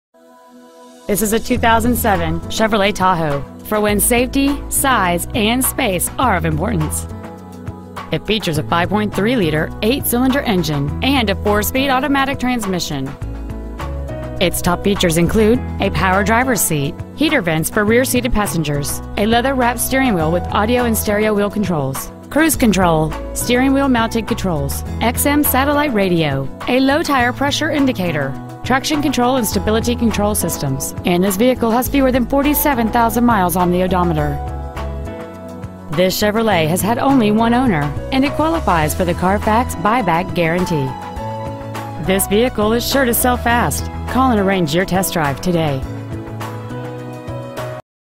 2007 Chevrolet Tahoe Cumming GA
Year: 2007
 Make: Chevrolet
 Model: Tahoe
 Engine: Gas/Ethanol V8 5.3L/323
 Trans.: Automatic
 Exterior: Summit White
 Miles: 46,894
 Interior: Ebony

 Preowned 2007 Chevrolet Tahoe Cumming GA